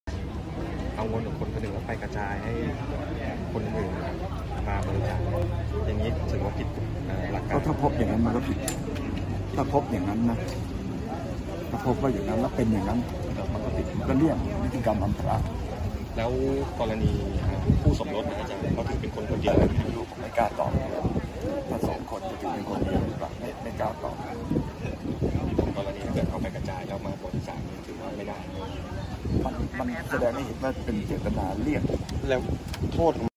คู่สมรสที่บริจาคให้พรรคการเมือง จะถือเป็นคนเดียวกันหรือไม่นั้น ตนไม่ทราบ - คมชัดลึก
1 พ.ย.2562 - ทำเนียบรัฐบาล นายวิษณุ เครืองาม รองนายกรัฐมนตรี กล่าวถึงกรณีมีการนำเงินตัวเองไปกระจายให้บุคคลอื่นบริจาคให้พรรคการเมืองเพื่อหลบเลี่ยงกฎหมายที่กำหนดให้แต่ละคนบริจาคได้ไม่เกิน 10 ล้านบาทต่อปี ถือว่าผิดหรือไม่ ว่า ถ้าพบอย่างนั้นและเป็นอย่างนั้นมันก็ผิด เป็นนิติกรรมอำพราง

มันแสดงให้เห็นว่าเป็นเจตนาเลี่ยง ขณะที่โทษร้ายแรงหรือไม่นั้น ตนจำไม่ได้ ไม่เคยดูเลยเรื่องนี้ ส่วนกรณีคู่สมรสที่บริจาคให้พรรคการเมือง จะถือเป็นคนเดียวกันหรือไม่นั้น ตนไม่ทราบ จึงไม่กล้าตอบ

คู่สมรสบริจาคพรรรค, วิษณุ เครืองาม, รองนายกรัฐมนตรี